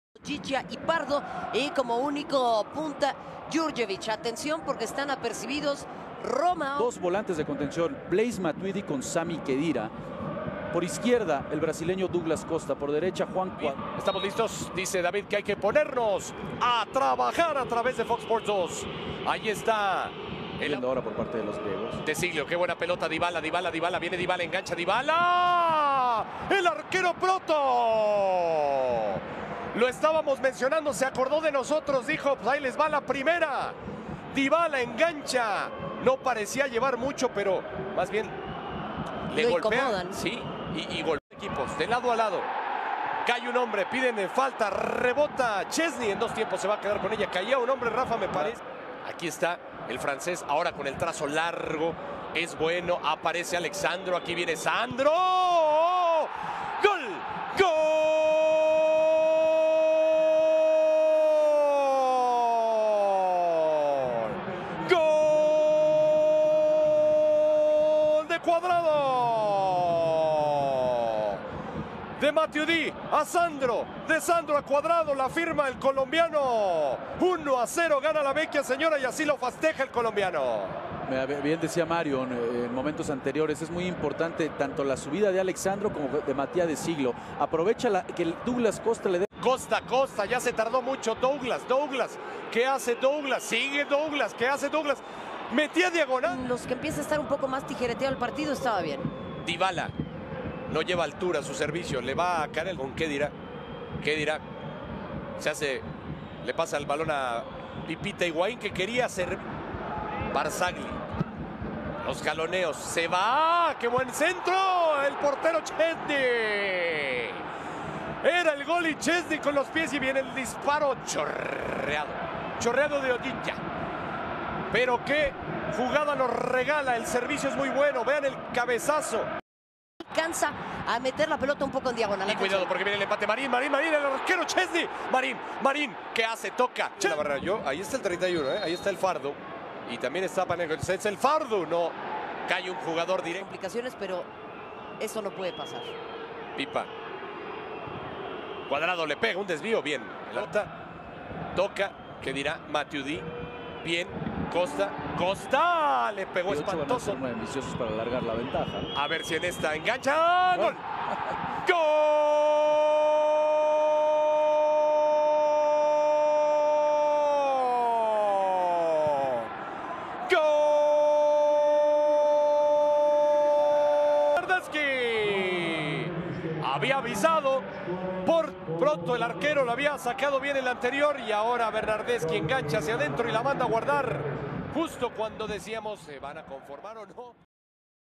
Olympiacos vs Juventus 0-2 All Goals and Highlights UCL 05-Dec-2017 HD
Olympiacos vs Juventus 0-2
All Goals and Highlights
UCL 05-Dec-2017 HD

➥About Channel
Confederations Cup,UEFA ,Top Goals,Funny Moments and Tricks and Tricks